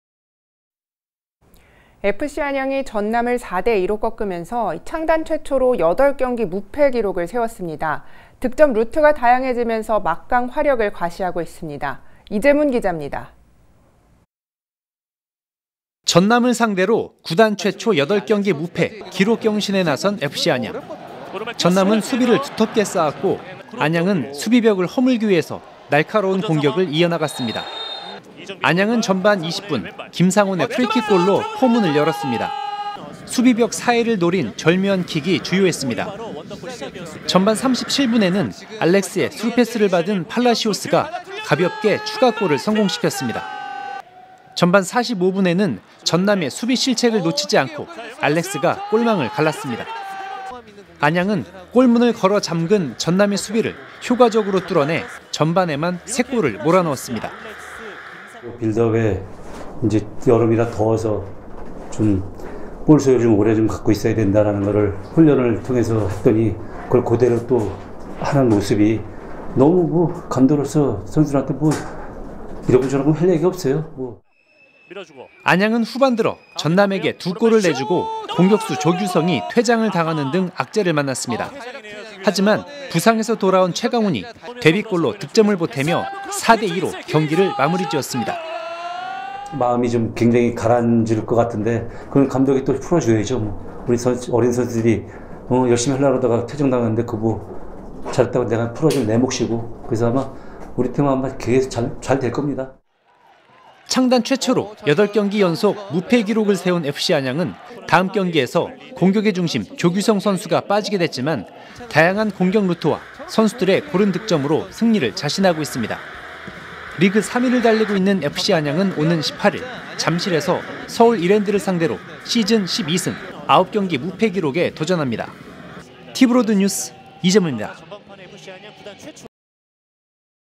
[안양] FC안양 구단 최초 리그 8경기 연속 무패 기록! / VS 전남 4:2 승리!
FC안양이 전남을 4:2로 꺾으면서
창단 최초로 8경기 연속 무패 행진을 이어갔습니다.